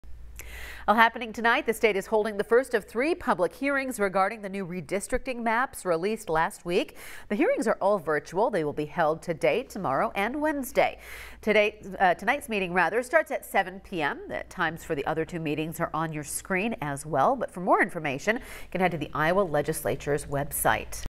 State to hold first redistricting public hearing Monday night
Iowans will be able to voice their opinions about the proposed redistricting maps during virtual meetings on Monday, Tuesday and Wednesday this week.